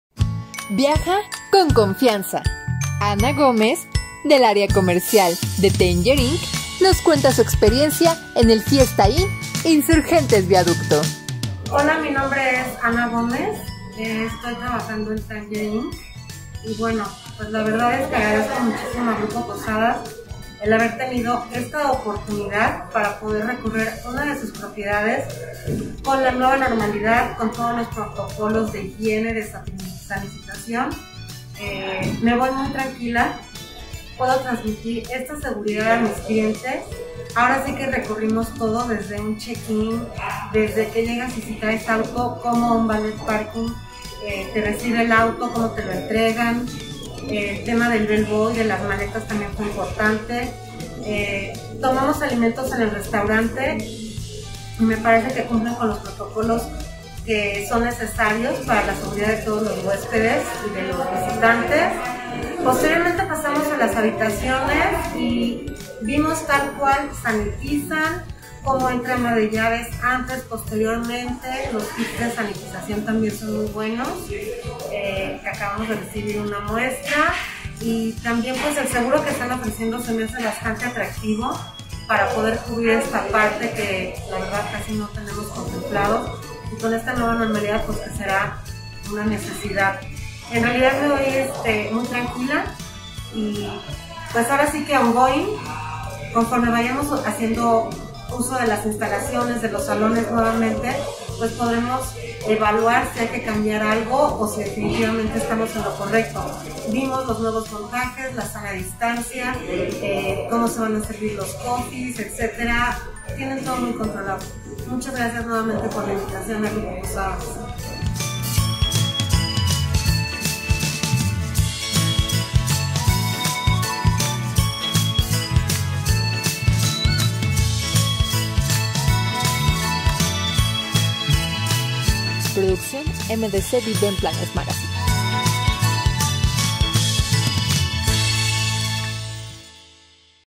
¡Viaja con Confianza!
Ana Gómez, de la empresa Tanger-Inc Meetings & Events, nos cuenta su experiencia en el hotel Fiesta Inn Insurgentes-Viaducto.